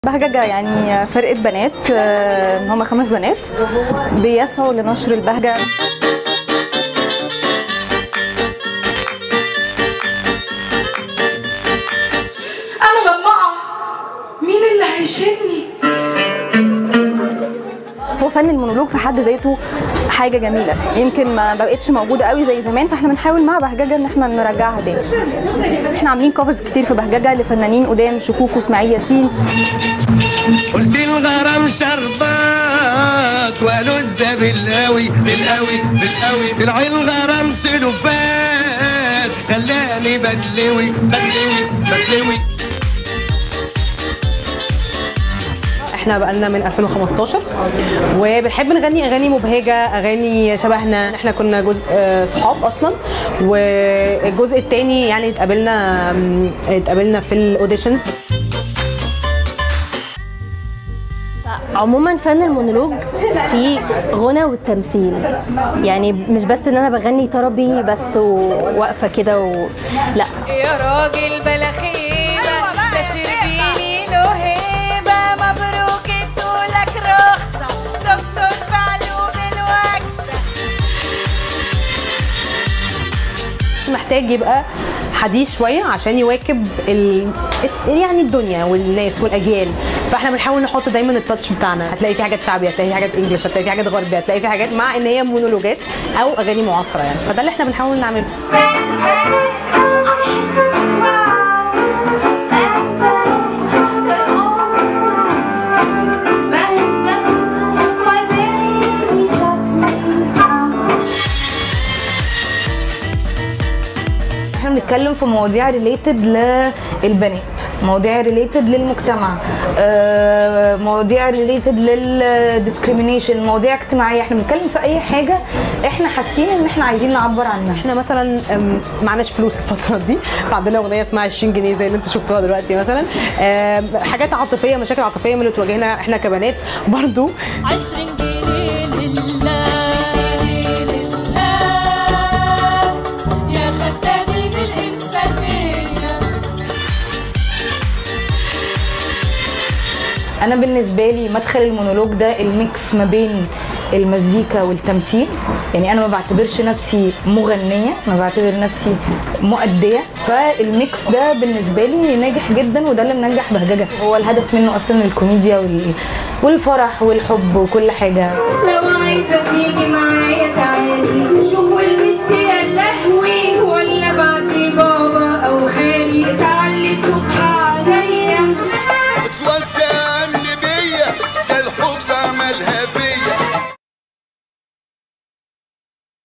"بهججة" ملكات المونولوج المصري 🤩
واضح يلتقي "بهججة" ملكات المونولوج المصري 🤩" ، فرقة مكونة من 5 شابات مصريات يرغبن في نشر البهجة من خلال المونولوج! 🤩🎤

Wada7 Original